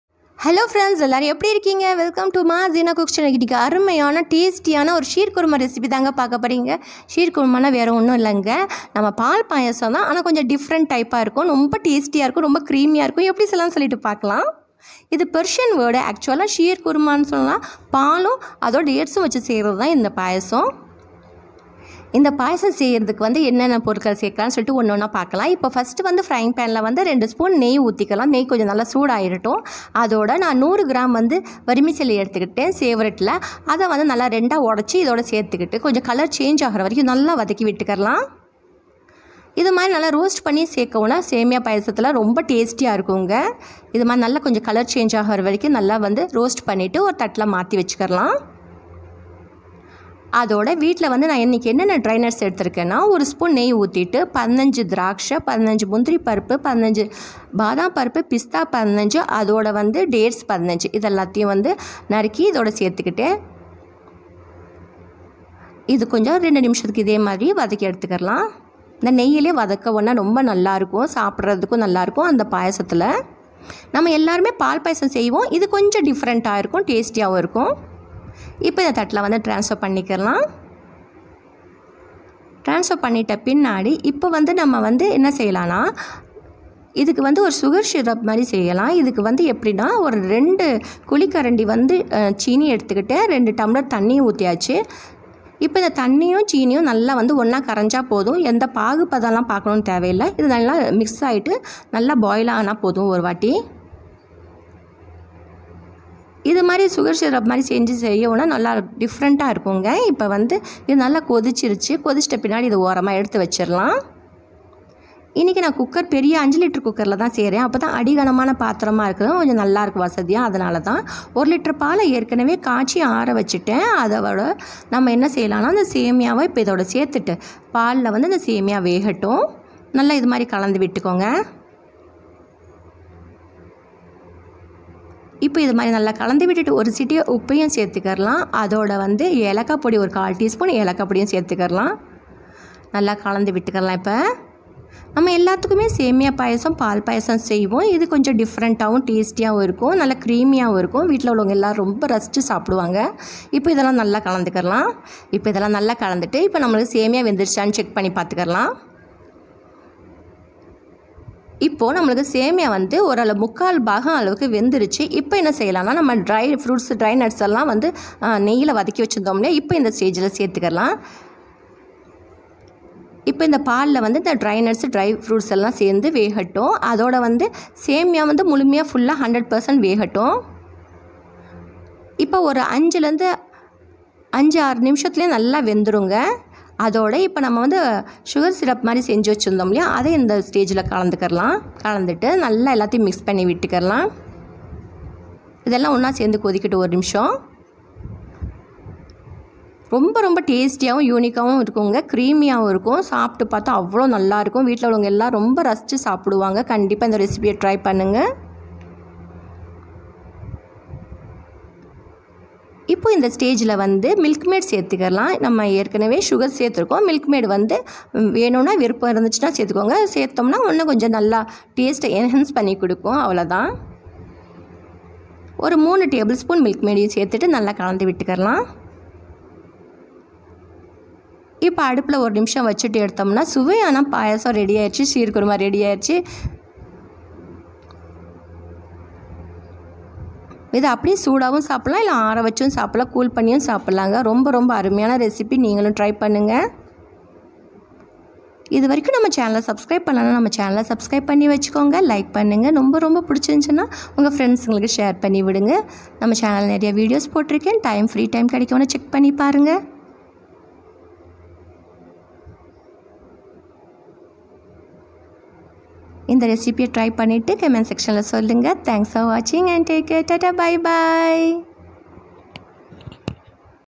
பால் பாயாசம் ஒரு முறை இது போல் செய்து பாருங்கள் 😋
vermicelli 100 gm
milk 1 litre full fat
Dry fruits- Dates,badam, cashew nuts, pistachio and raisins each 15 nos chopped
 Try it friends hope you like it...